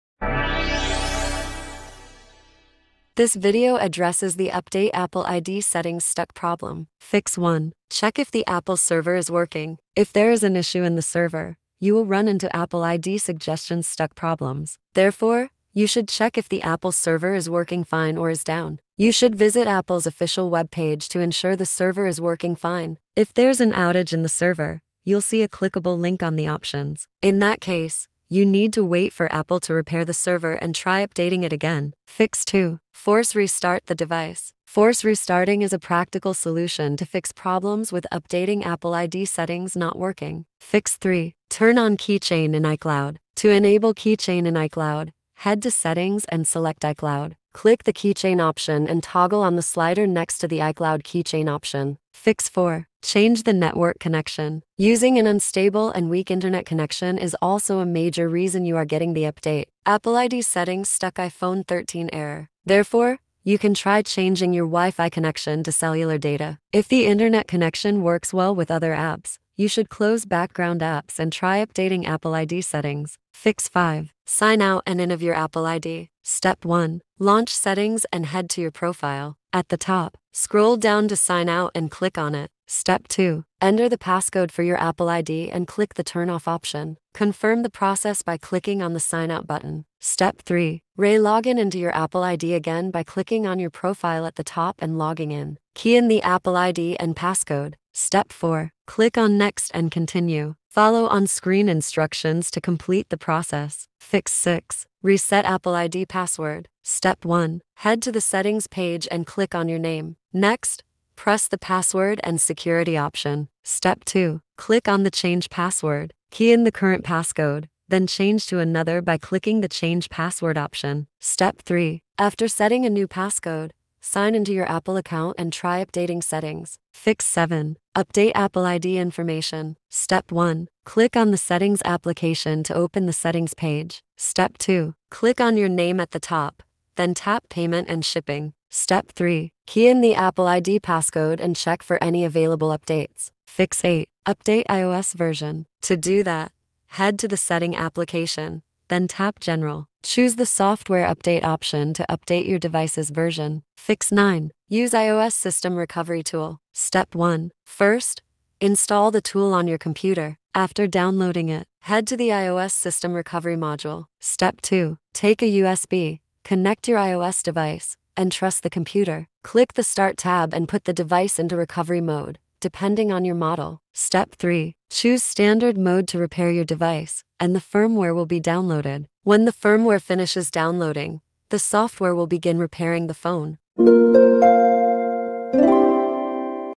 How to Fix Updating Apple ID Settings Stuck Issue
Updating Apple ID settings get stuck? Fret not. Here are some easy ways to get rid of the problem.